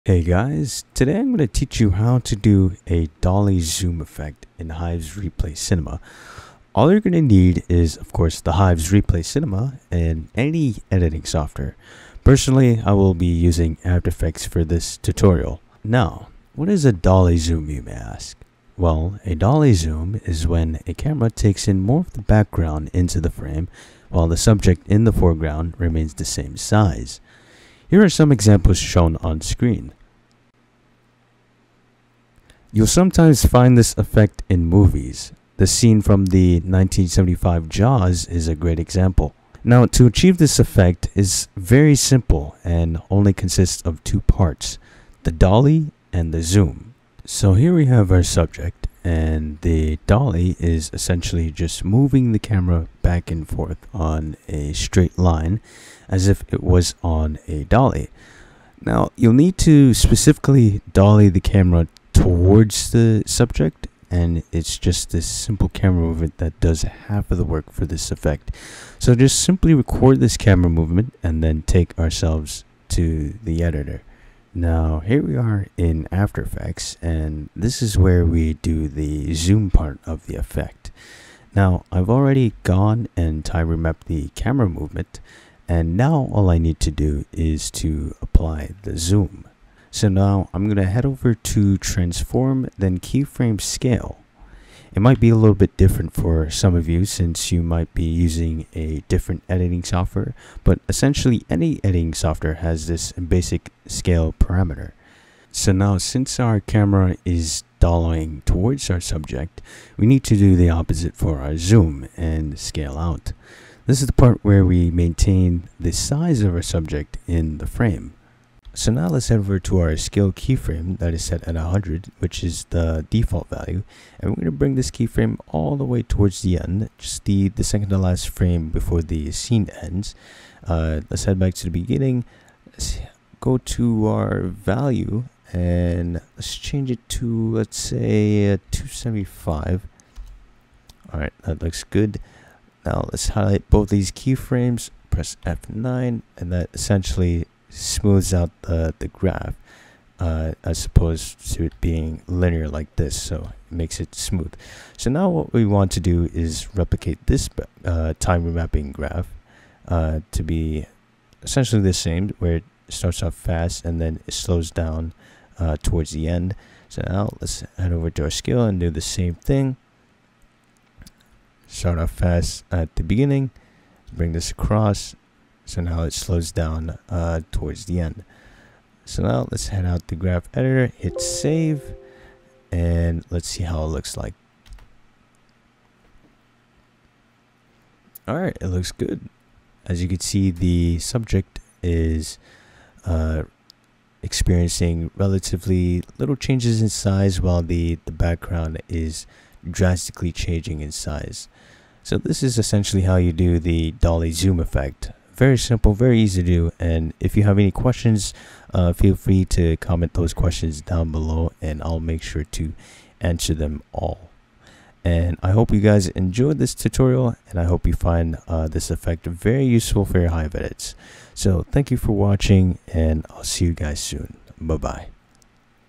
How To Do A Dolly Zoom Effect In Hive's Replay Cinema
A short in depth tutorial on how to do the dolly zoom effect in Hive's Replay Cinema.

HEC Co-Owners:
AstroMEC - @astromecmc
MICO - @MICOhive